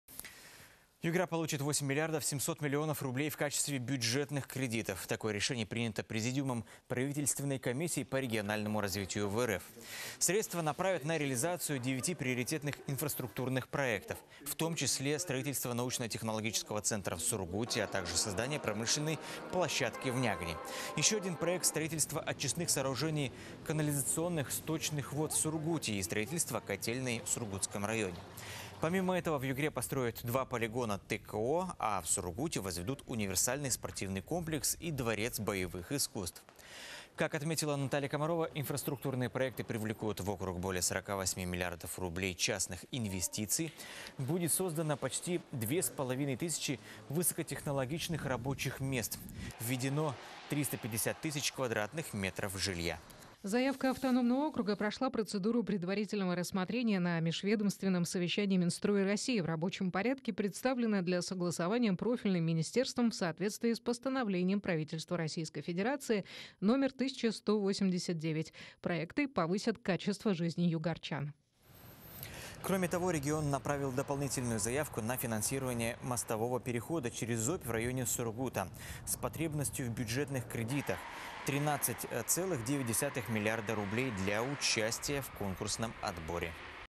Югра получит более 8 миллиардов рублей на крупные стройки
Югра получит 8 миллиардов 700 миллионов рублей в качестве бюджетных кредитов. Такое решение принято президиумом правительственной комиссии по региональному развитию в РФ.